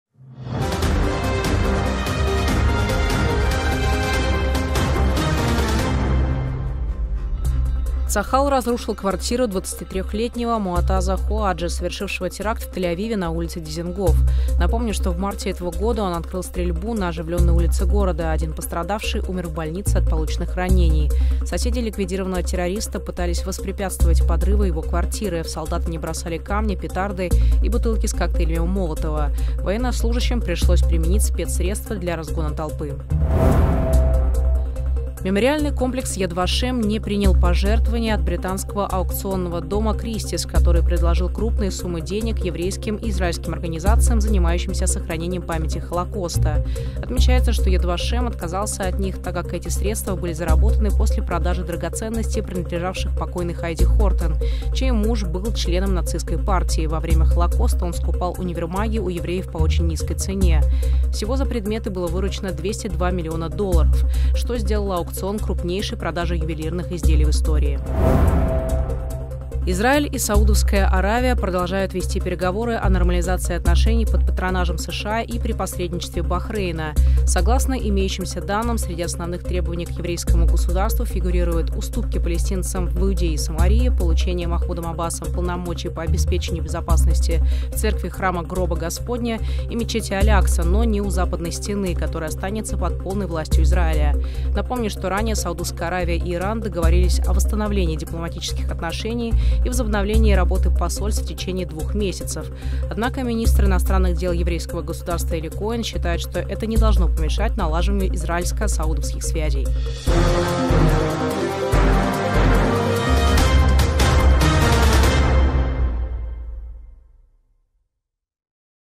Израиль и Саудовская Аравия сближаются | Скандал с имуществом нацистов | НОВОСТИ ОТ 23.05.23
- ЦАХАЛ разрушил квартиру террориста, убившего еврея
- Аукцион Christie's продал драгоценности с нацистским прошлым
- Израиль и Саудовская Аравия начали процесс сближения